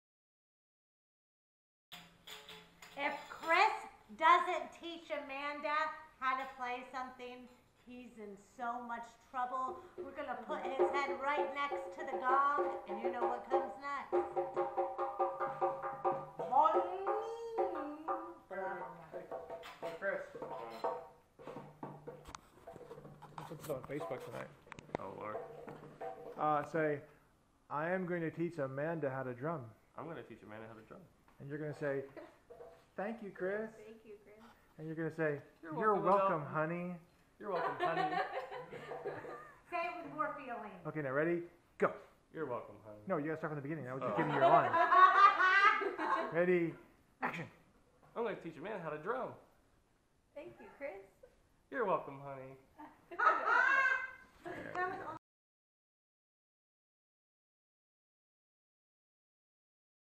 Chris Promising to Teach Amanda How to Drum - Tribal Soup Project
So after a number of times of pushing Chris to start teaching his love soon to be wife to drum more we now have it documented on video of his promise, of course we had to cue him up a little bit but it was still kind of funny.